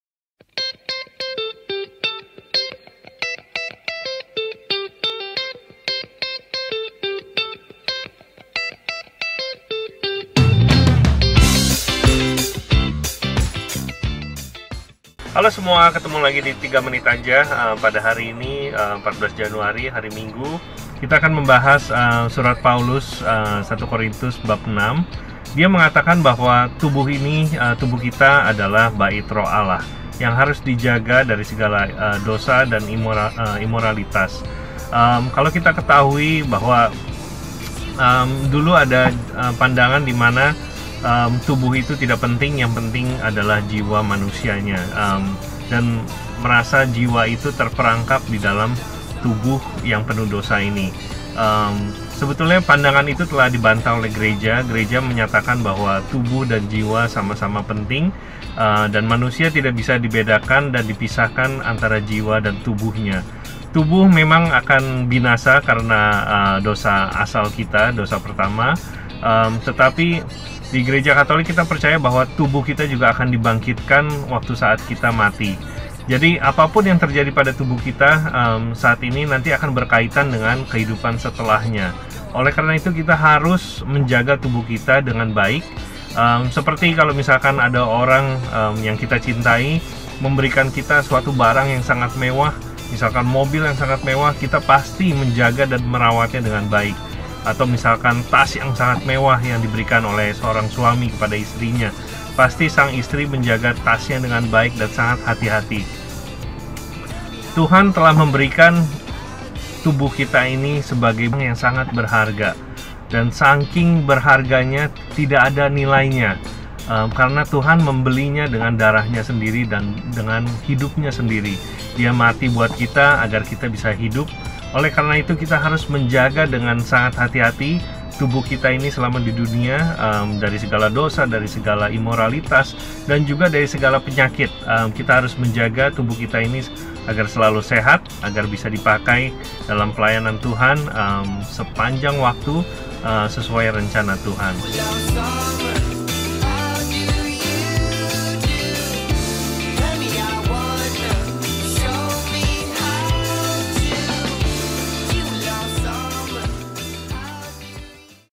3MenitAja | Jagalah Hati + Jagalah Tubuh | Ernest Basarah
Bacaan Minggu ini mau ngomong apa sih untuk hidup orang muda?

Bacaan Pertama: 1Samuel 3:3b-10,19
Mazmur Tanggapan: Mazmur 40:2,4ab,7-10
Bacaan Kedua: 1Korintus 6:13c-15a,17-20
Bacaan Injil: Yohanes 1:35-42

Ada pandangan yang bilang kalau tubuh itu nggak penting, yang penting cuma jiwa aja. Padahal St. Paulus bilang kalau tubuh adalah bait Roh Kudus. Gimana sih yang bener?

INSPIRE YOUNG PEOPLE
Make better decisions in Christ